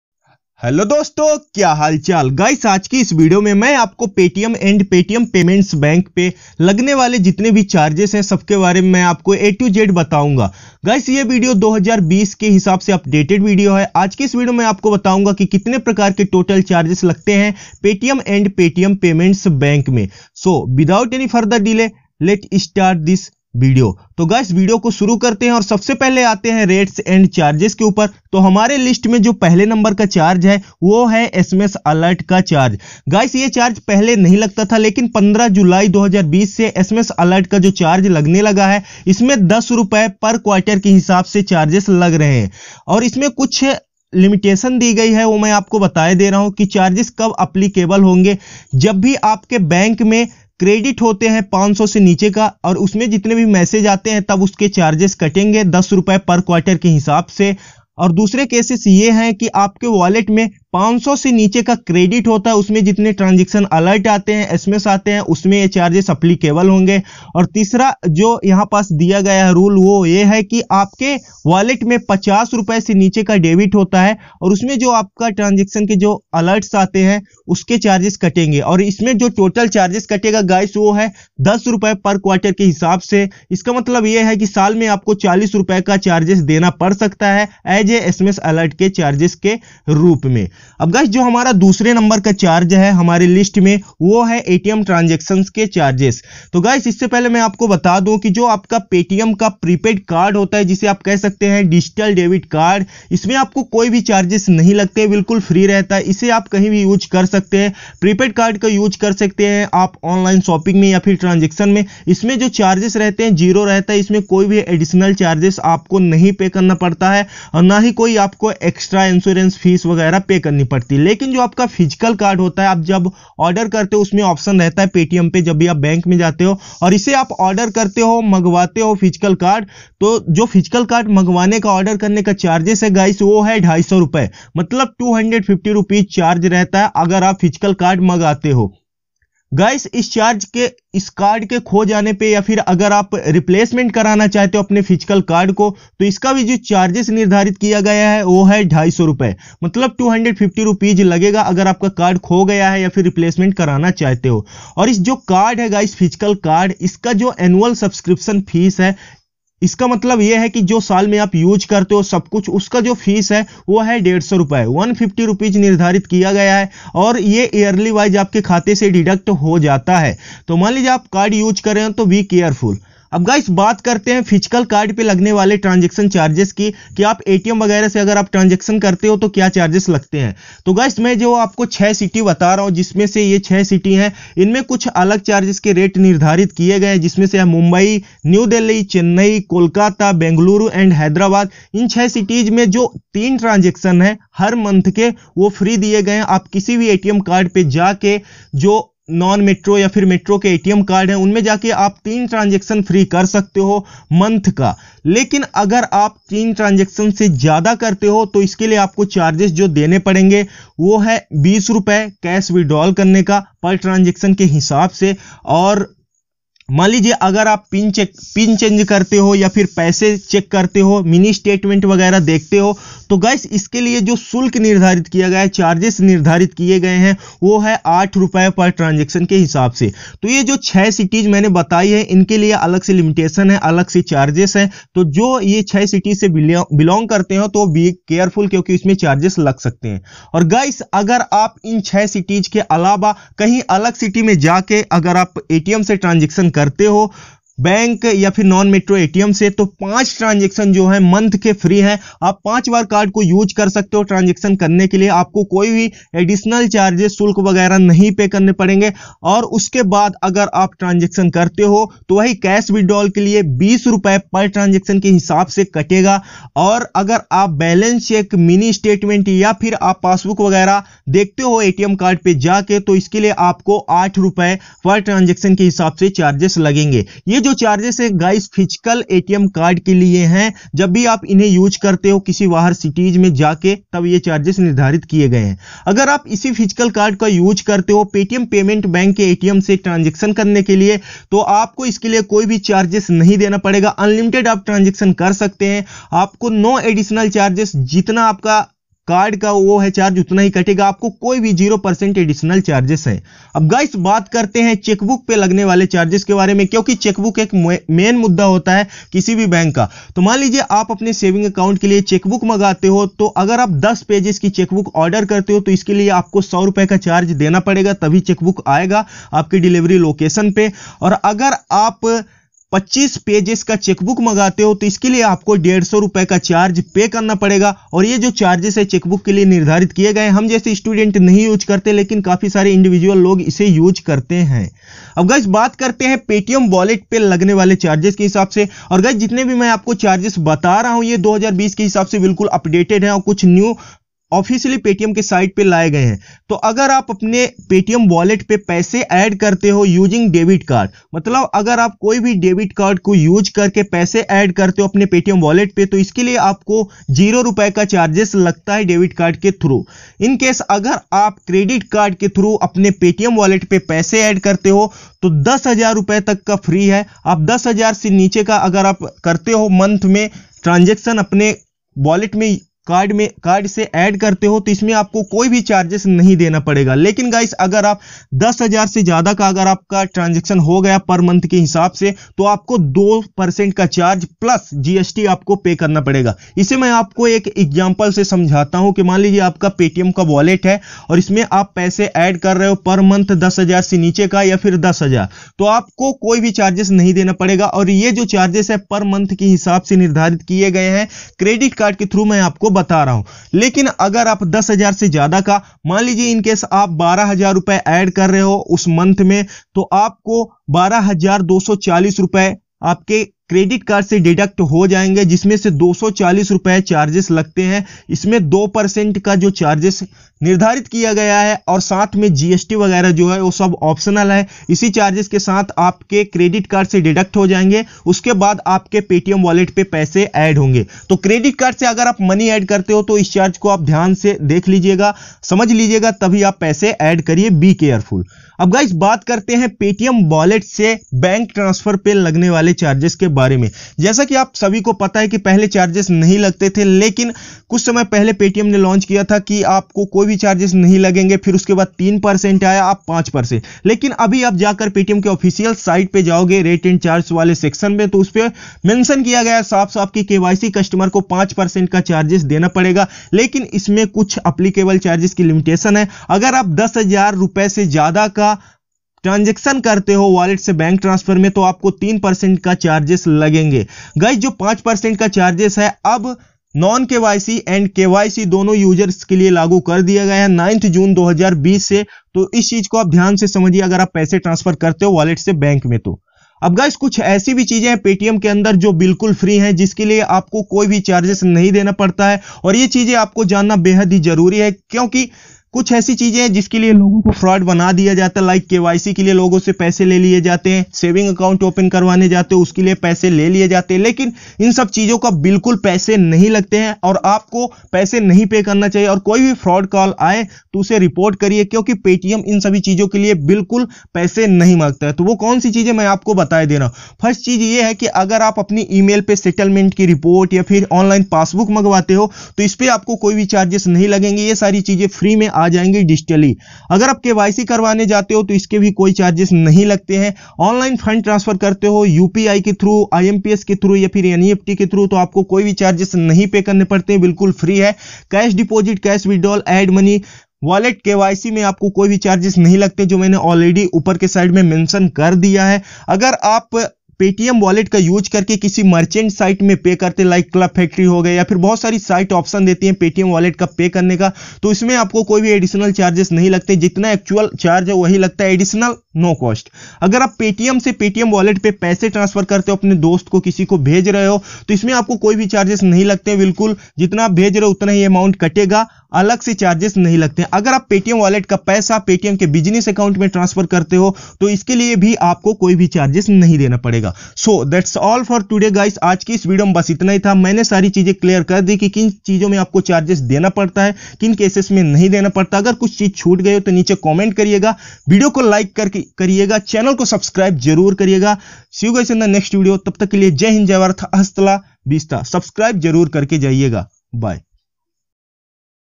Paytm Wallets And Paytm bank All Updated Charges List in 2020|Know all about of Paytm Charges
Hello dosto kya halchal In this video I have talked about Paytm charges and how can be save that charges, guys please watch this video, before using Paytm app in 2020, because you don't  know how many charges are in Paytm,
Paytm Wallets And Paytm  bank All Updated Charges List in 2020|How many types of Charges in Paytm | watch this video..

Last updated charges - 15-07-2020

Under 1,000
Under 2,000
My Mic
My Future Mic

My Editing Earphone
Headphones (I use)
Laptop (Similar to Mine)